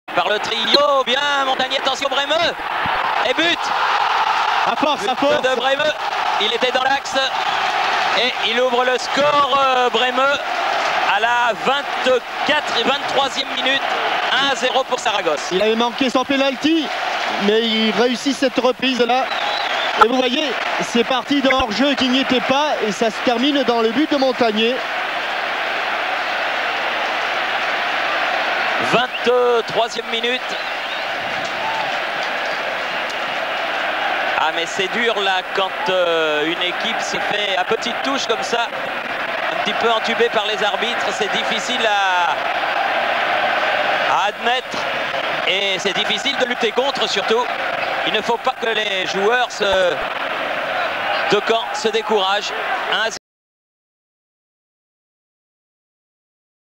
Copa Uefa 1992/93.Gol de Andreas Brehme.Real Zaragoza - SM Caen
1/10/1992. 1/32 Copa de la UEFA. Partido de vuelta. La Romareda. Real Zaragoza-SM Caen. Gol de Andreas Brehme que abría el marcador para el conjunto zaragocista. (2-0 fue el resultado final del partido y el conjunto blanquillo remontaba la eliminatoria)